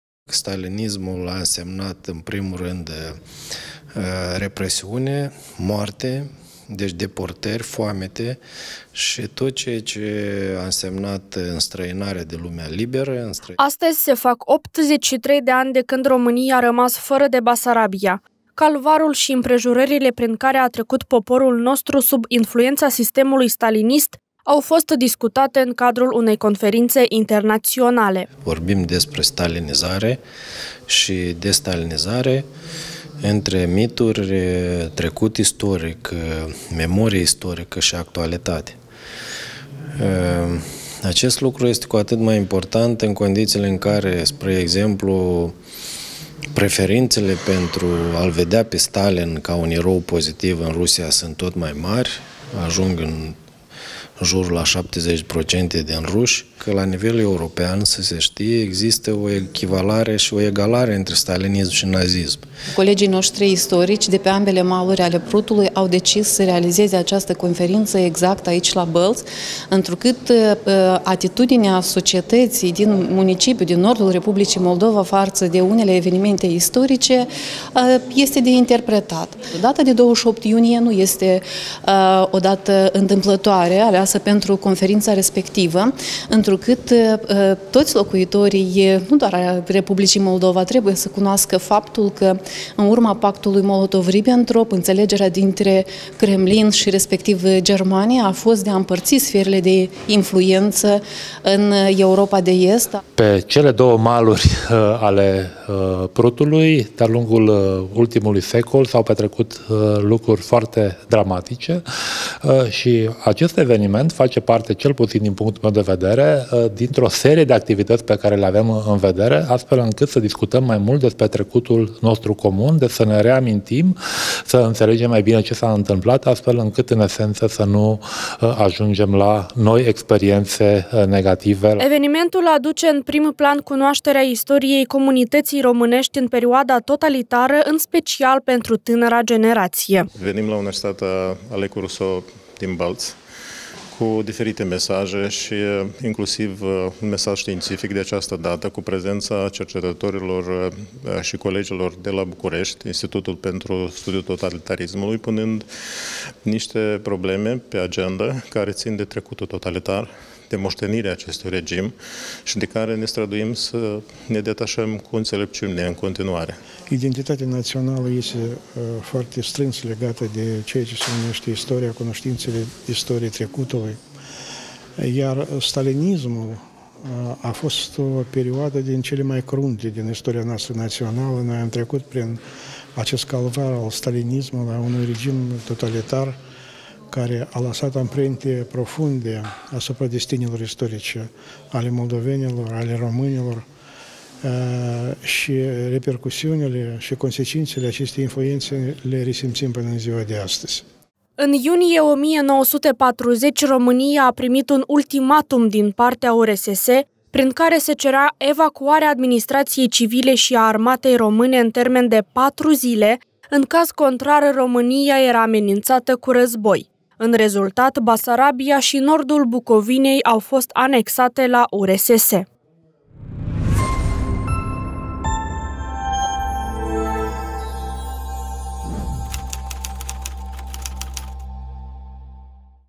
STALINIZAREA ȘI DESTALINIZARE, DISCUTATE LA USARB
Astăzi se fac 83 de ani de când România a rămas fără de Basarabia. Calvarul și împrejurările prin care a trecut poporul nostru, sub influența sistemului stalinist a fost discutat în cadrul unei conferințe internaționale.